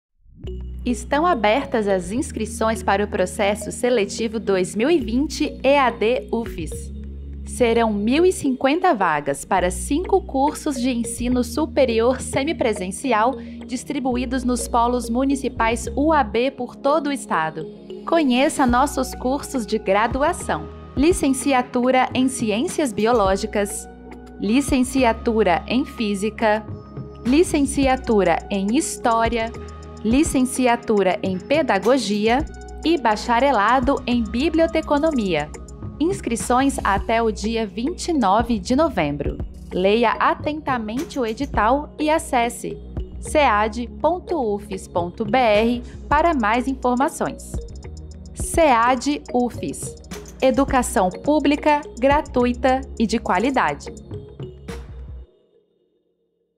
Processo Seletivo 2020 EaD Ufes
Processo Seletivo para ingresso no curso de Bacharelado em Biblioteconomia e nos cursos de Licenciaturas em Ciências Biológicas, Física, História e Pedagogia, na modalidade de ensino a distância, para o primeiro semestre de 2021.